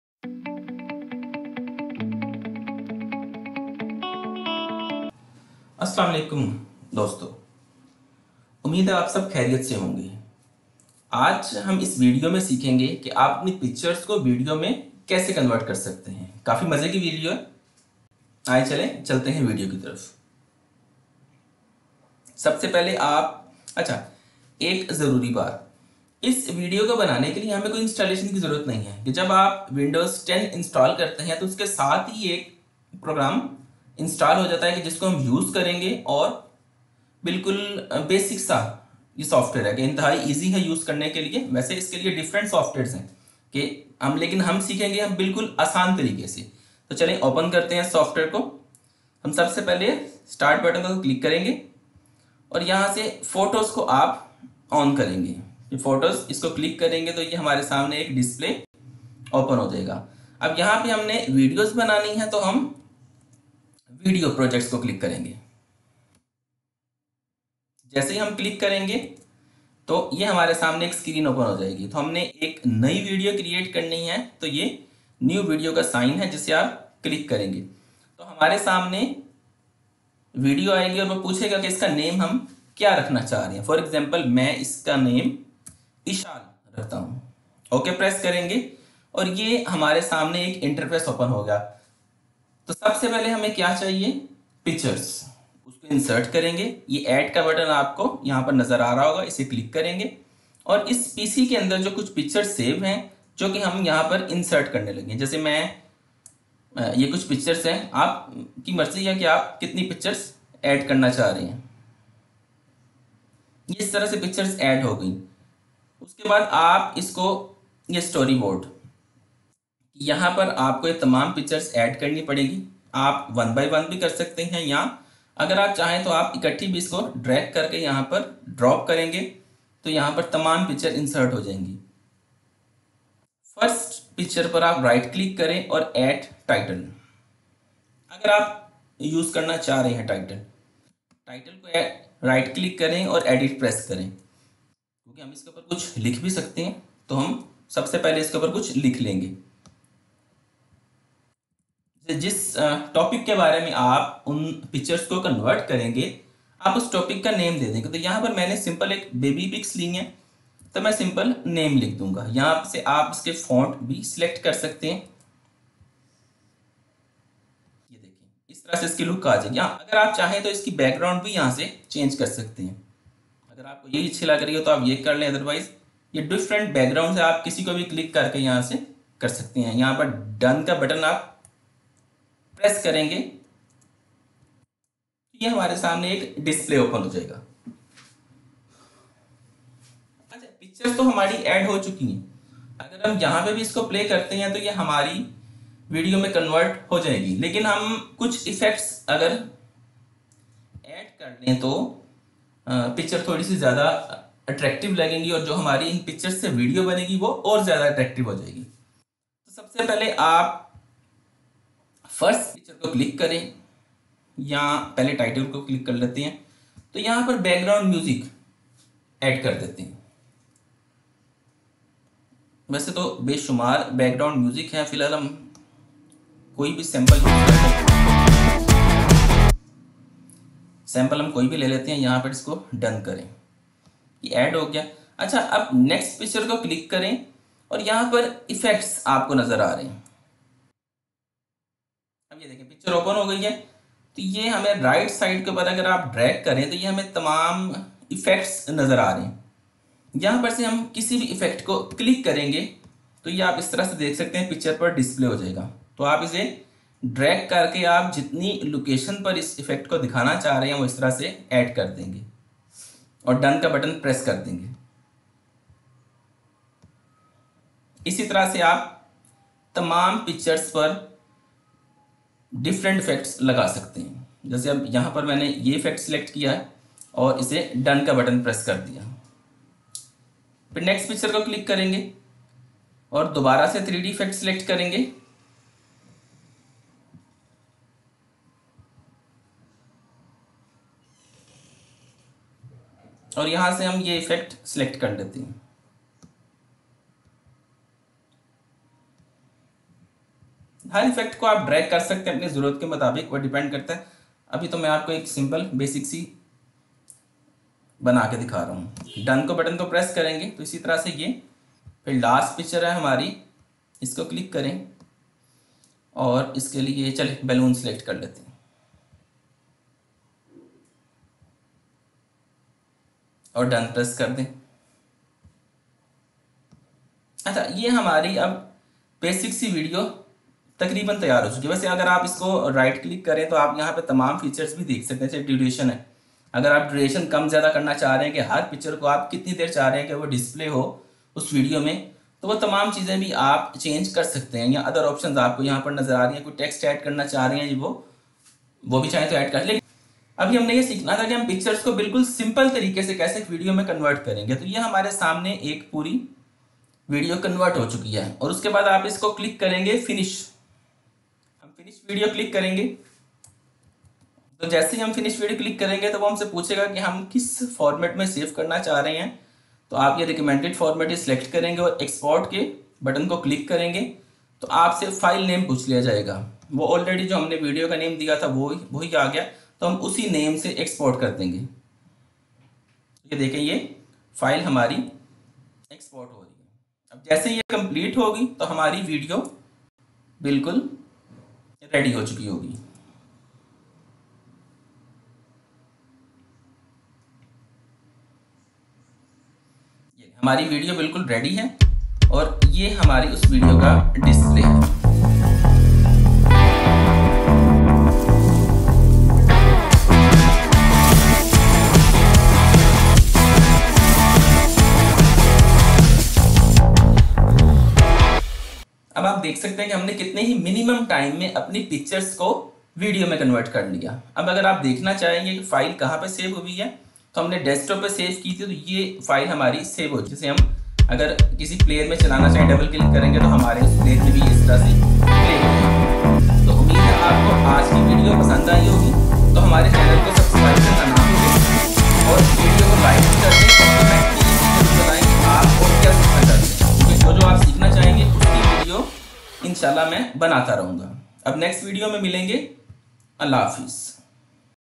How to Convert Pictures Into a Video from Microsoft Photos | Photos to Video maker
Watch How to Create Video with Pictures...  Make a Movie of Pictures Easily.